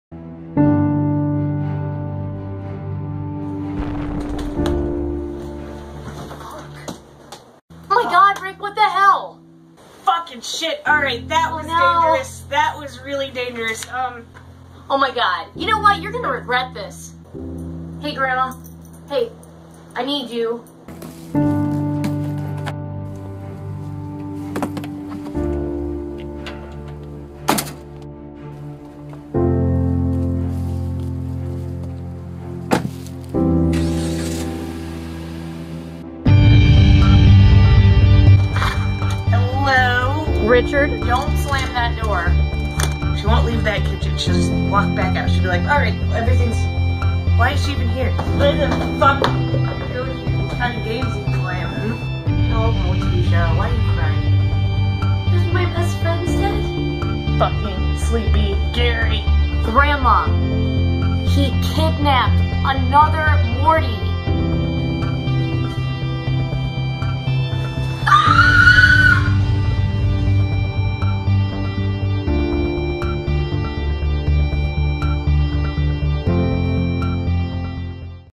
Preview of Episode 105 The Truth | Live Action Adventures of Rick and Morty
TO CLARIFY!!! THIS IS NOT THE LAST EPISODE OF THE SERIES!!!
Info on the LIVESTREAM!
There will be a livestream at 10pm EST on Friday December 1st after episode 105 The Truth premieres. Please join us for that!

all characters of Rick and Morty belong to Justin Roiland, Dan Harmon and Adult Swim.